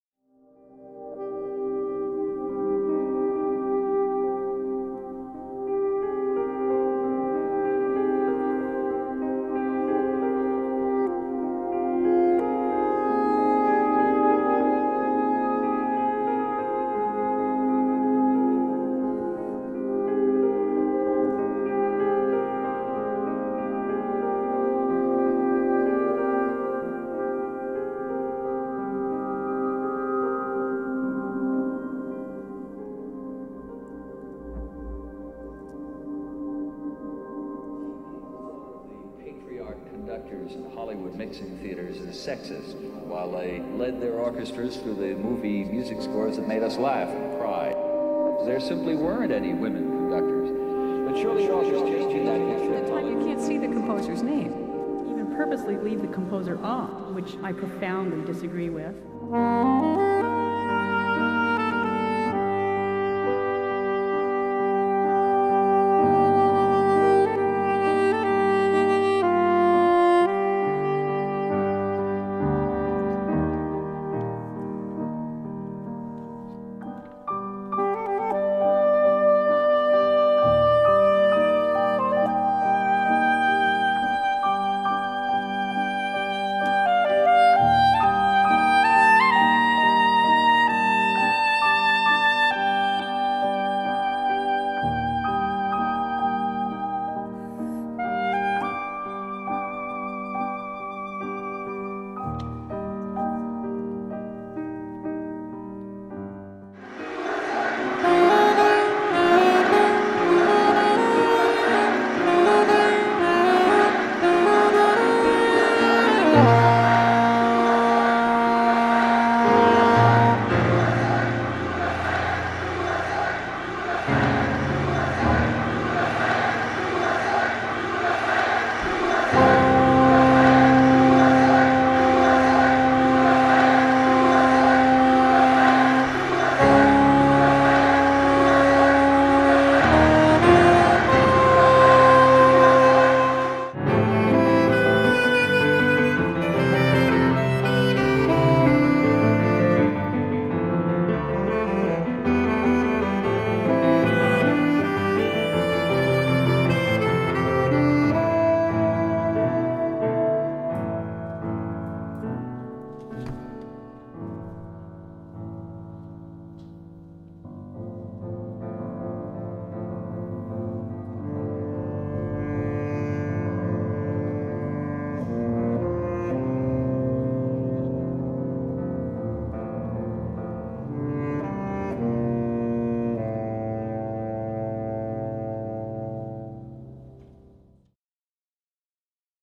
Screen & Sax 2017 montage
Montage of the Screen and Sax II programme at Kings Place, London, as part of Equator Festival's Women of the World series, featuring works by British female composers including Jennie Muskett, Rachel Portman OBE, Debbie Wiseman MBE, Anne Dudley. Jocelyn Pook, Lisa J Burgess and Charlotte Harding.

Saxophones: Hannah Marcinowicz
Piano: Philip Mountford